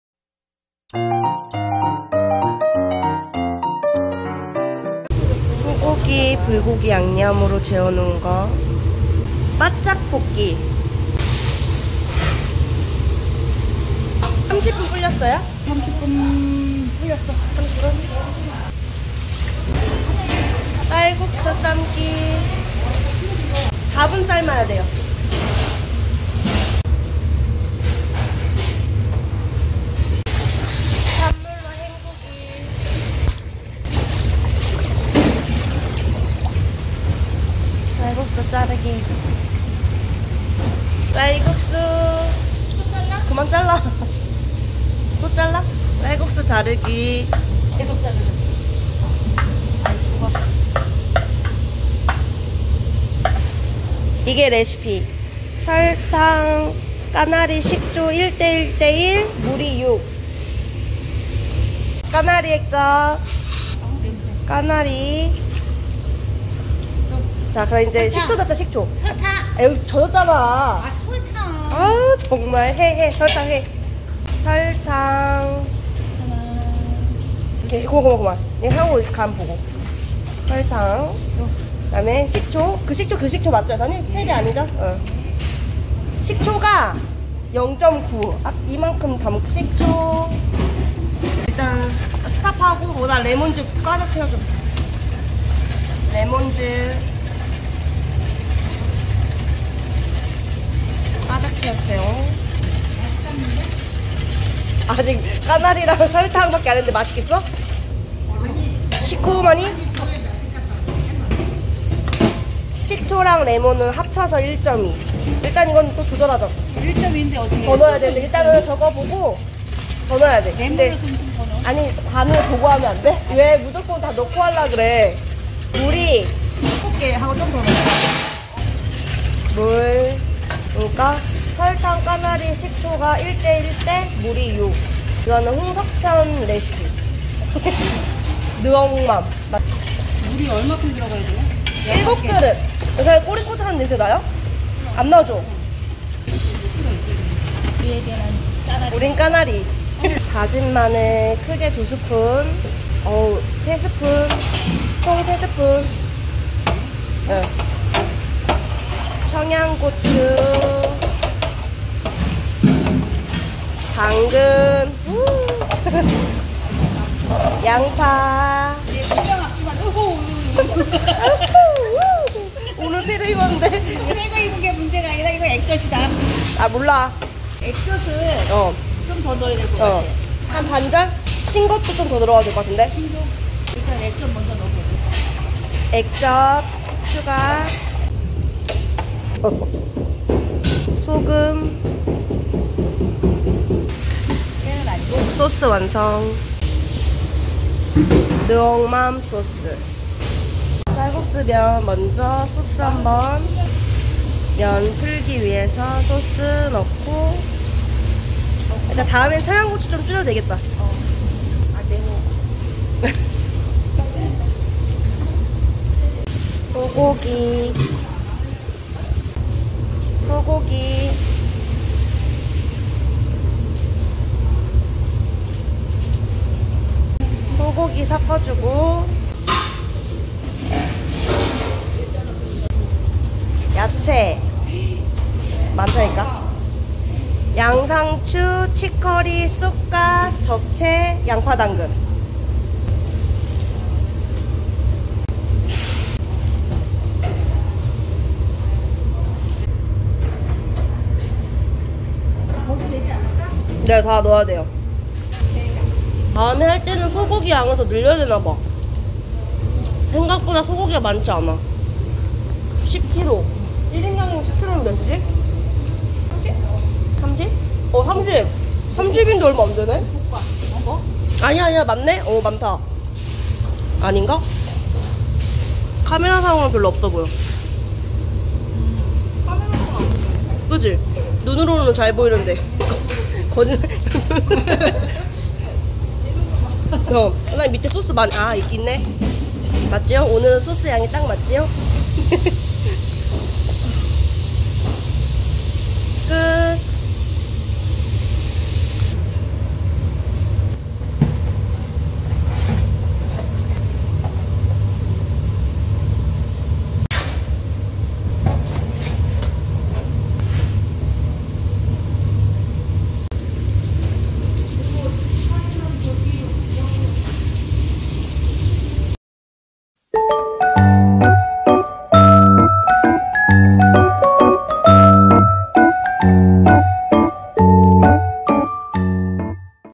단체급식(대용량):불고기 분짜샐러드(Bulgogi Bun Cha Salad)만들기♡느억맘소스
★불고기 분짜샐러드 310인분 레시피(대면배식)★

쌀국수(건면,3mm 250g, 몬라이스누들) 22봉(5.5kg)
양상추 5kg
치커리 1.5kg
쑥갓 1.5kg
적채 1.5kg
양파 1.5kg
당근 1kg

(1인량:쌀국수17g, 양상추16g, 치커리4~5g, 쑥갓4~5g, 적채4~5g, 양파5g, 당근3g)

1.소불고기 바짝 볶고, 식히기! (간장,설탕,다진마늘,후춧가루,미향넣고 양념에 재워놓음)
★소불고기는 고명이라 작게 잘라서 사용했어요.

2.쌀국수는 찬물에 30분 불린 후, 끓는 물에 4분 정도 삶고 찬물로 여러 번 헹구기!
★쌀국수 면이 길면 배식할 때 힘들어서, 저희는 작게 잘라주었어요.

3.느억맘소스 만들기!
(홍석천 레시피 참고함 설탕,까나리액젓(or피쉬소스), 식초 1:1:1 비율)

★피쉬소스를 사용하려고 했는데, 향이 강할 것 같아서 까나리액젓 사용했어요. 저희 업장은 식초+레몬즙을 혼합해서 사용했어요.

4.살짝 굳어(붙어) 있는 쌀국수 면을 풀어주기 위해서, 느억맘소스 넣고 풀어주기!

5.식혀 놓은 소불고기 넣고 섞어주기!

6.채소 넣고 남은 느억맘소스 넣고 버무려주기!

7.완성!!